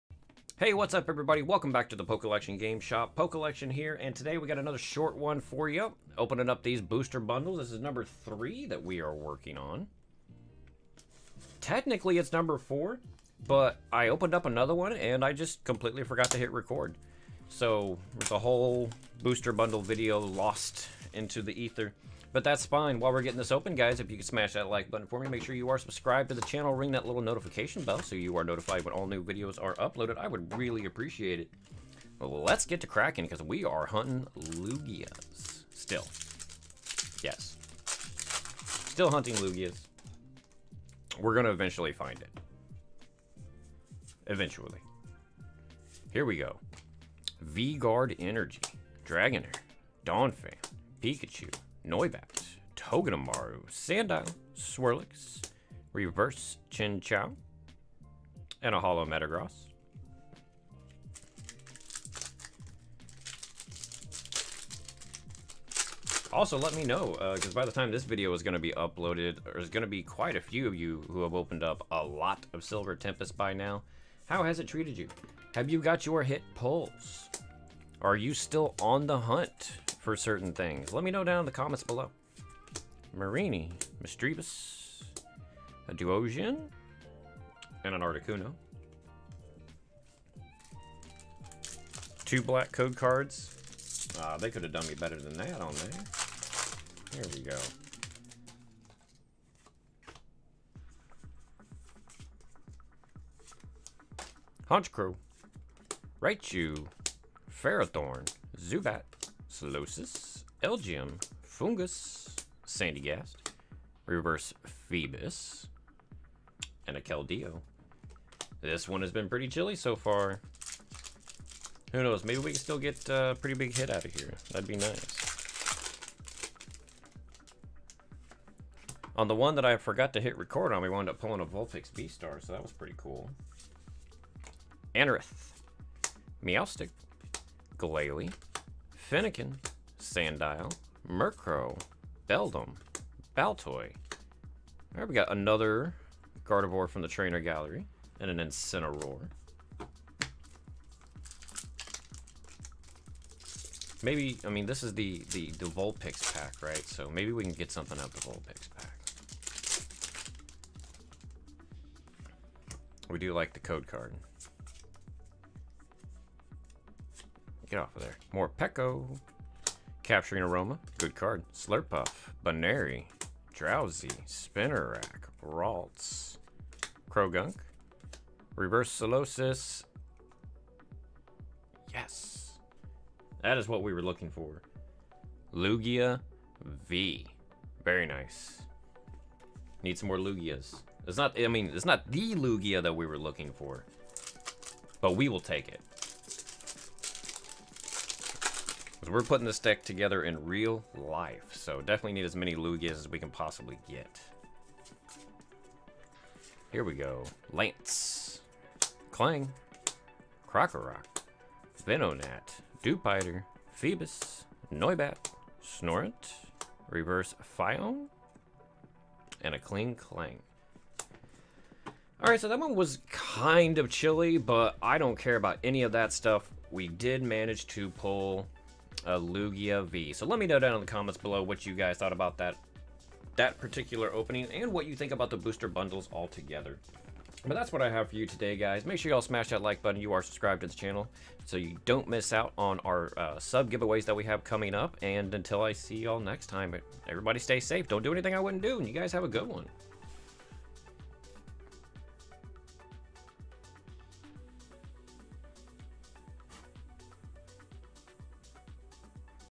Pokemon Silver Tempest Booster Bundle Opening #3
Add me in the PTCGO: pokellection285
Add me in Pokemon GO: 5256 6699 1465

Today I am opening up another Silver Tempest Booster Bundle!